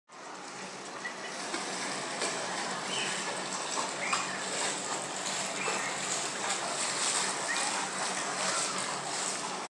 Tuesday Time Trial at Knysna Canoe Club lap 1
The run through the harbour of Tuesday's time trial held by the Knysna Canoe Club.